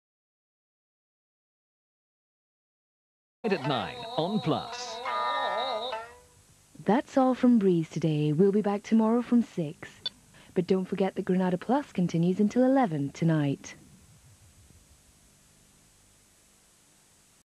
Granada Breeze - Closedown, 1998
A 6pm closedown of the defunct Granada Breeze channel, it then goes into a crashed version of the Cable TV Guide.